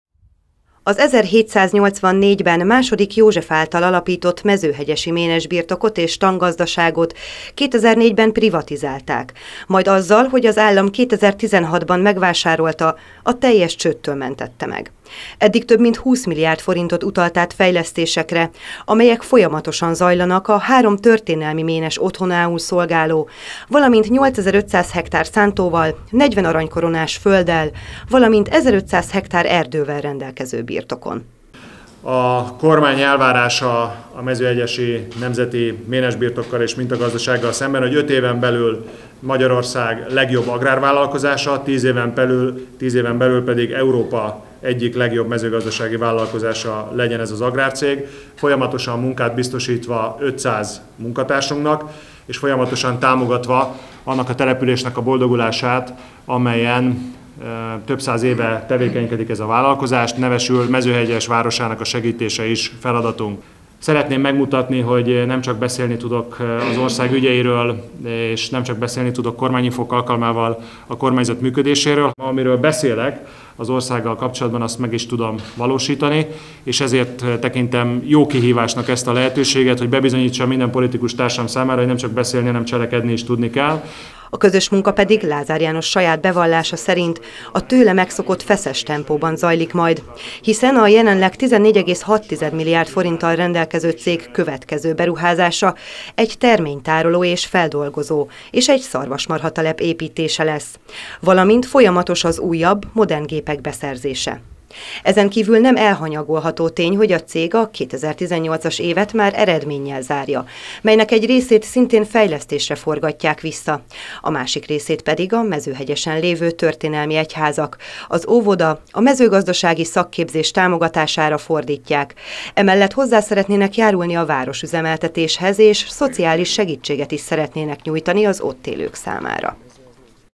Lázár János lett a mezőhegyesi ménesbirtok kormánybiztosa
„Azért is tekintem jó kihívásnak ezt a lehetőséget, hogy bebizonyítsam minden politikustársam számára: nem csak beszélni, hanem cselekedni is tudni kell” – emelte ki Lázár János legújabb megbízatása kapcsán, október 1-től ugyanis az állam képviseletében ő gyakorolja a nagy múltú mezőhegyesi Nemzeti Ménesbirtok és Tangazdaság tulajdonosi jogait. A célja pedig nem kevesebb, mint hogy a birtok öt éven belül Magyarország legjobb agrárvállalkozása, tíz éven belül pedig Európa egyik legjobb mezőgazdasági vállalkozása legyen.